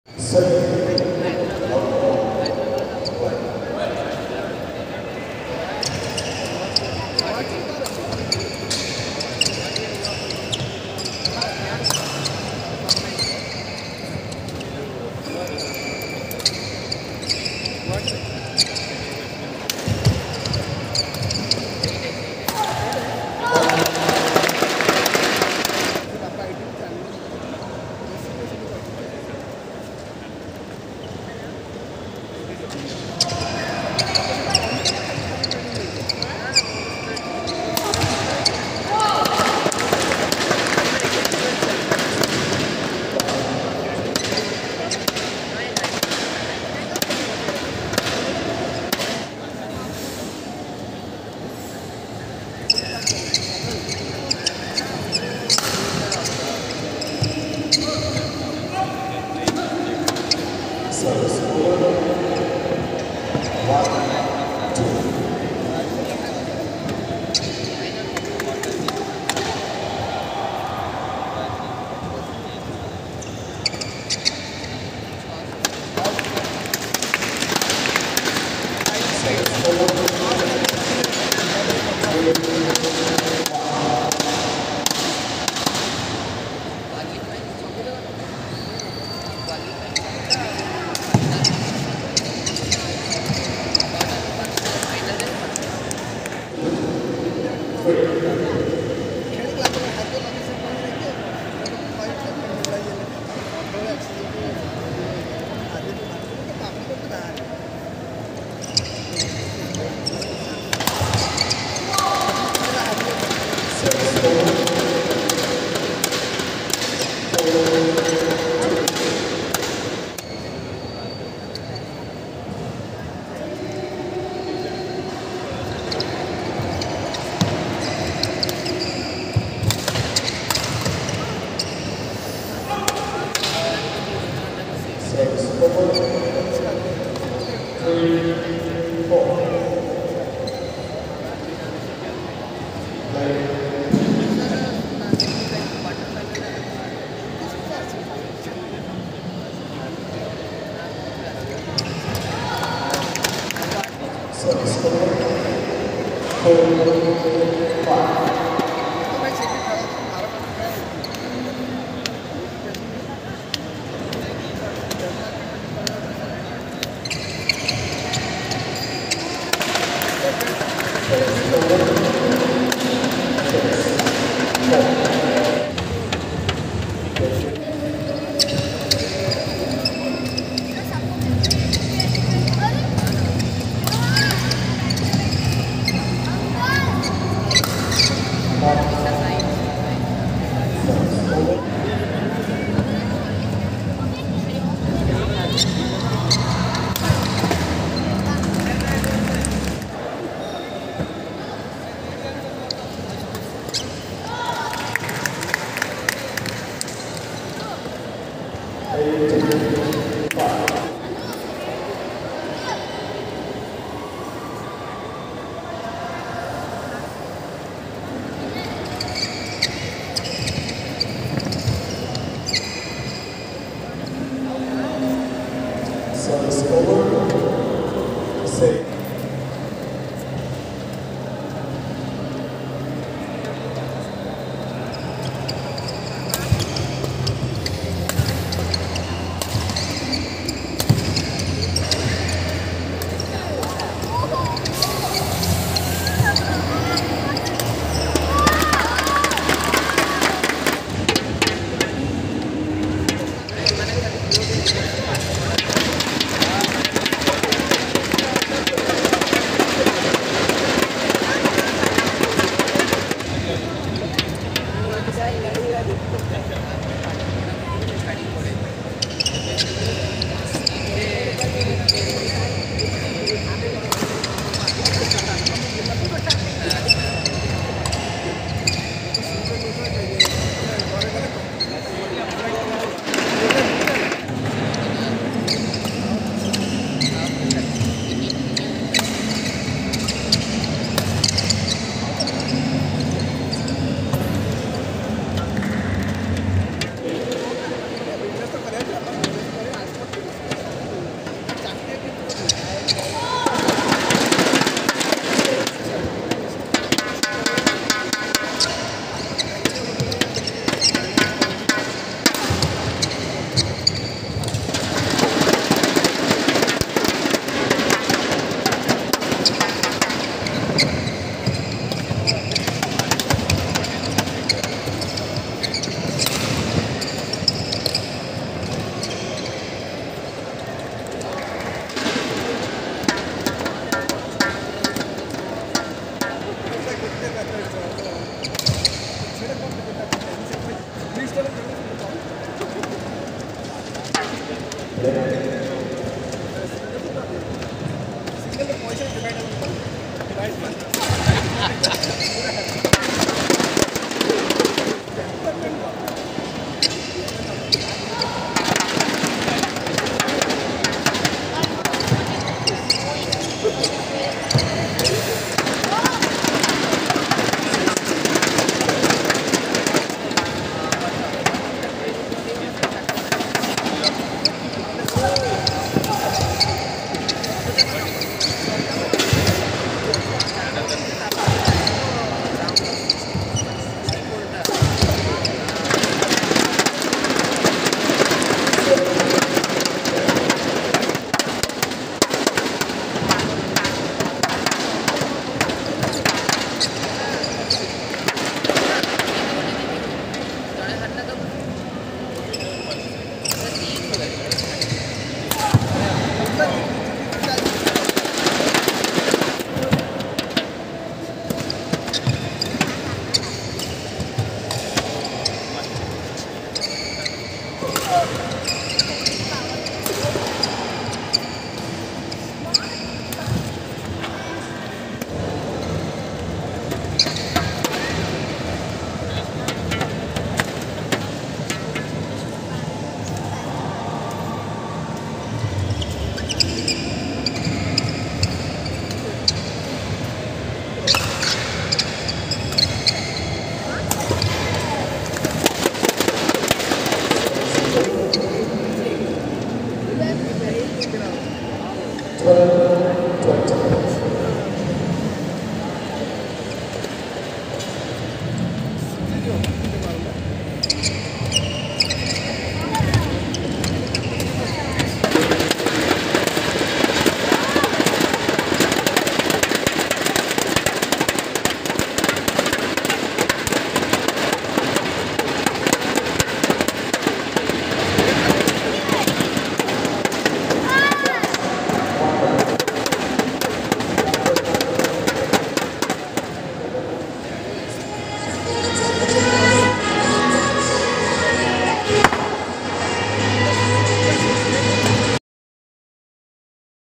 XD U19 Final All India | Bhargav Ram A.(AP)/Vennala K.(TS) vs Arsh M.(UP)/Manya R.(PNB) Set 2
Mixed Doubles U19 Final | Bhargav Ram Arigela (AP)/Vennala K. (TS) vs Arsh Mohammad (UP)/Manya Ralhan (PNB) Set 2 | Yonex Sunrise All India Junior Ranking Badminton Tournament 2024 held at Bhubaneswar (1 to 7 August, 2024)